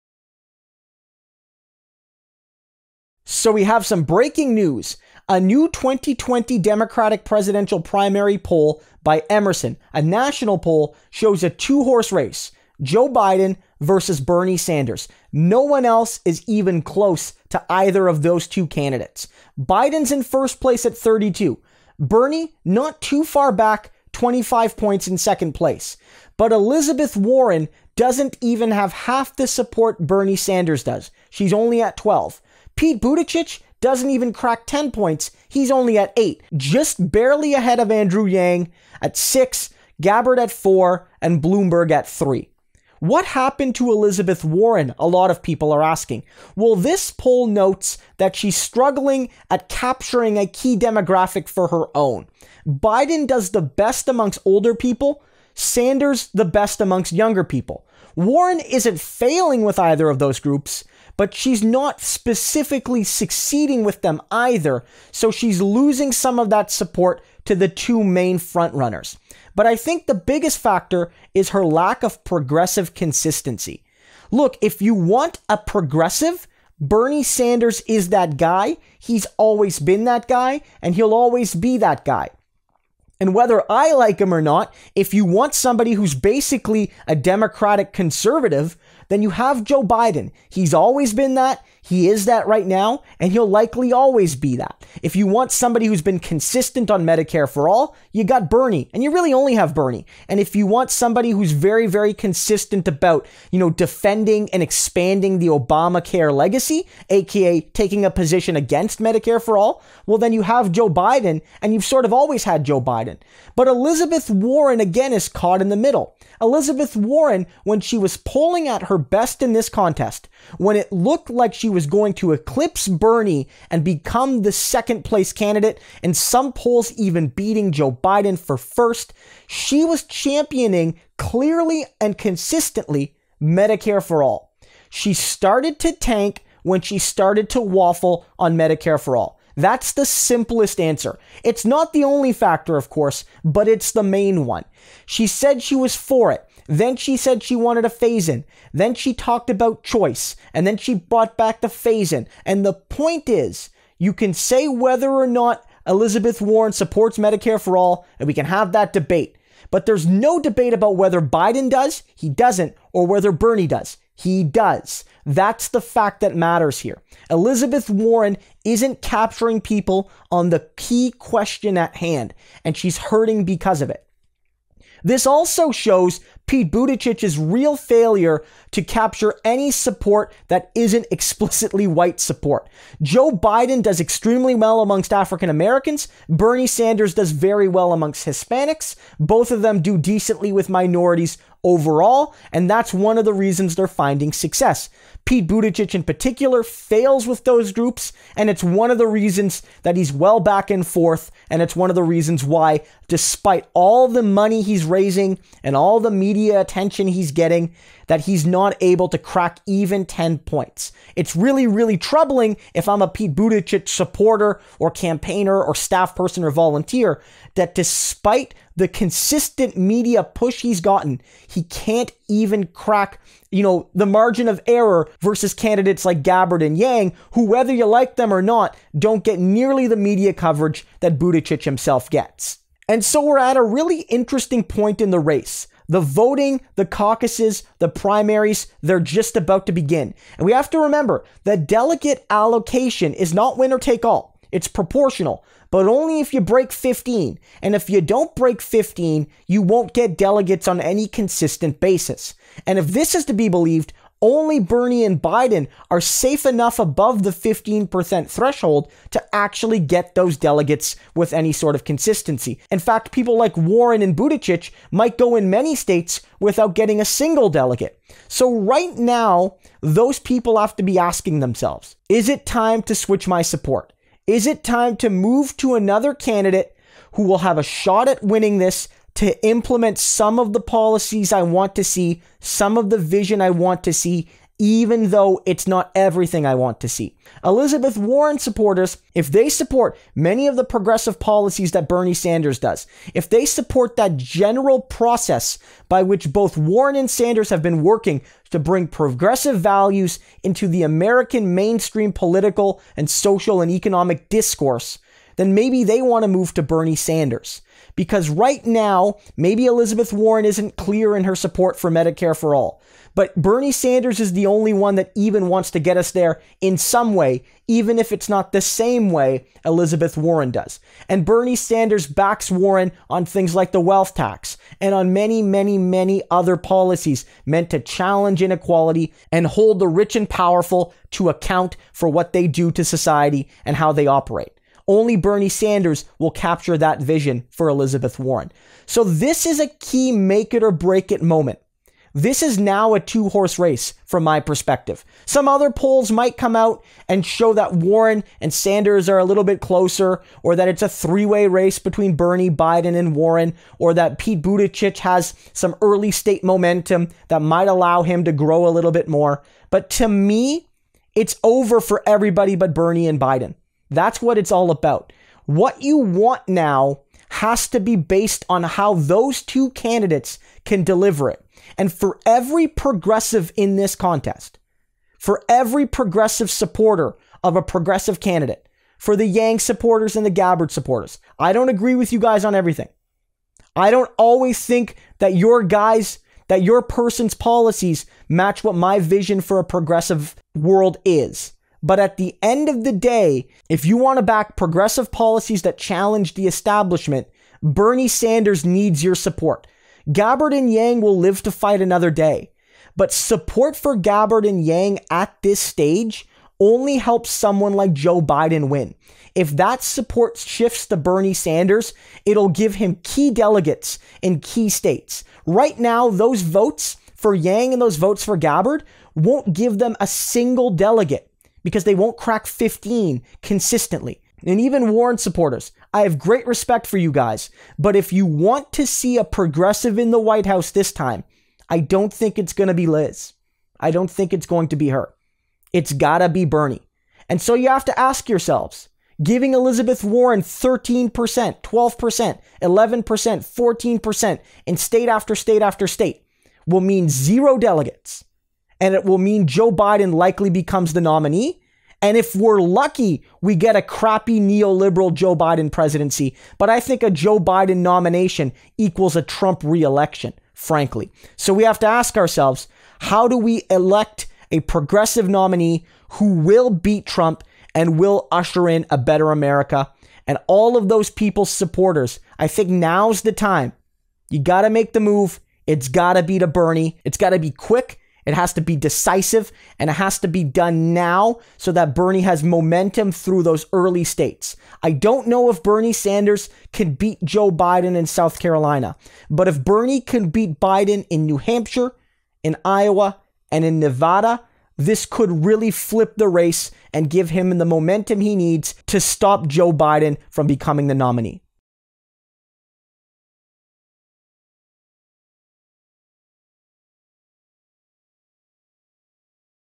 2020 Democratic Primary poll shows two horse race between Bernie Sanders and Joe Biden
A recent National Emerson poll shows that the 2020 Democratic Primary is a two horse race between Joe Biden and Bernie Sanders, with Elizabeth Warren falling way behind with less than half the support Bernie Sanders has. Pete Buttigieg has no real base of support, and sits below 10 percent.

The reason Pete is failing is because he has no real support among black or Hispanic voters, and Warren is tanking because she has been inconsistent on the key issue of Medicare for All, whereas Bernie Sanders and Joe Biden have been crystal clear, either for it or against it

As @TheMichaelBrooksShow has put it, it's time to stop fooling around and back Bernie Sanders 100%!

Here are the full results:
Biden 32%
Sanders 25%
Warren 12%
Buttigieg 8%
Andrew Yang 6%
Tulsi Gabbard 4%
Bloomberg 3%
Klobuchar/Booker/Steyer 2%

National 2020 Democratic Primary poll shows two horse race between Bernie Sanders and Joe Biden